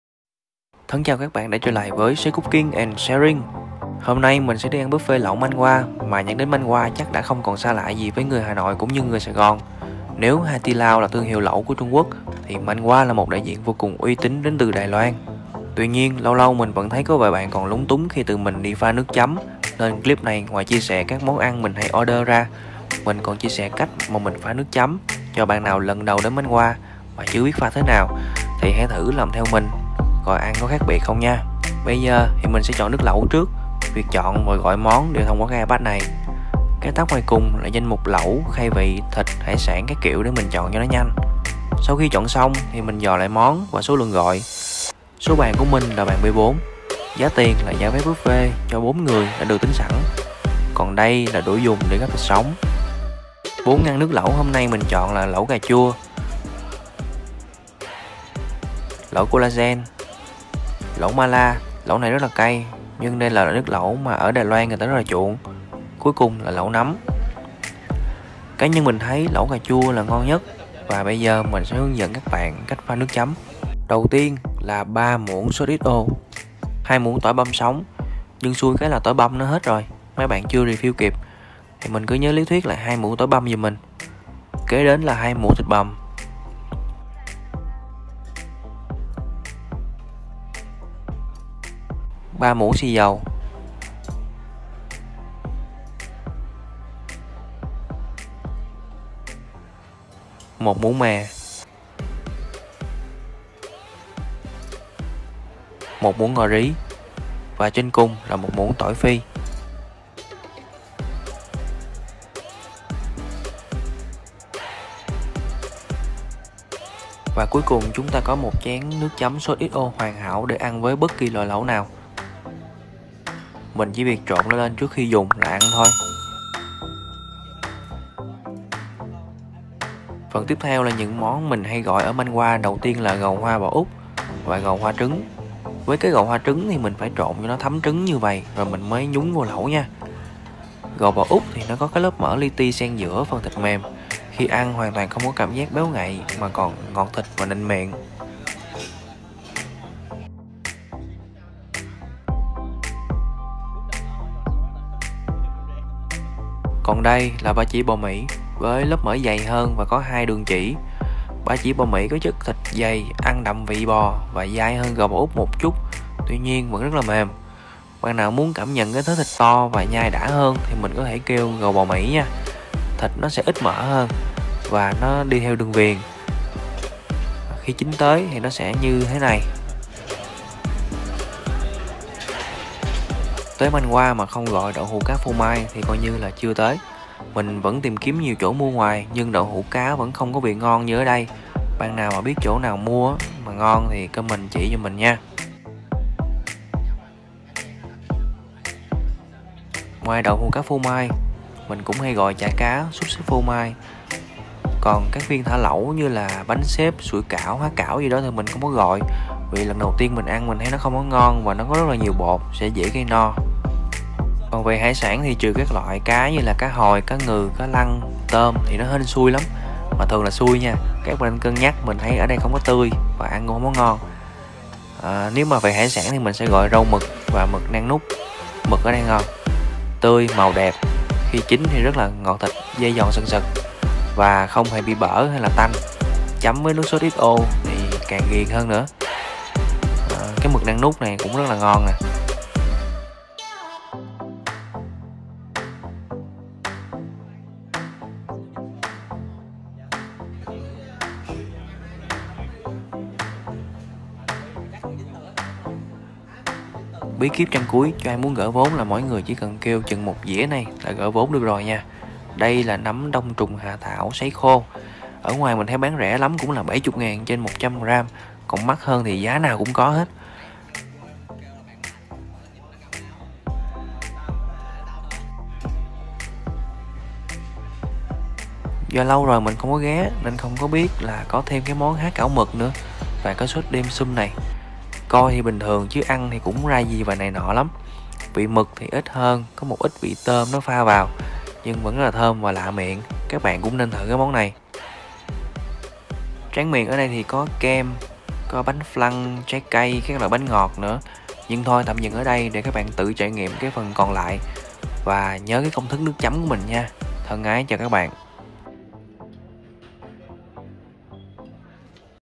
Cách Pha Nước Chấm Lẩu Manwah và Top Món Mình Hay Gọi Khi Ăn Buffet Lẩu Manwah TPHCM | Say Cooking
Buffet Lẩu Manwah không chỉ quen thuộc với giới trẻ Sài Gòn mà còn đang mở rộng thêm nhiều tỉnh thành khác như Manwah Hà Nội, Manwah Cần Thơ, Manwah Bình Dương, Manwah Biên Hòa.
Tuy nhiên trong nhiều lần đi ăn, mình cũng thấy nhiều bạn vẫn còn lúng túng không biết cách pha sốt chấm lẩu Manwah như thế nào nên hôm nay mình làm một video ngắn hướng dẫn bạn công thức của bản thân mình.
👉👉👉 Nếu bạn có công thức nào mới và ngon, hãy đóng góp thêm ở phần comment bạn nhé ^^
Địa chỉ mình hay đi:
Manwah Cao Thắng Mall, số 19 Cao Thắng, Phường 2, Quận 3
Mở cửa: 11:00 - 22:00 tất cả các ngày
Giá Vé từ 449K / PAX - Chưa bao gồm VAT và Phí Phục Vụ
Thân ái !!!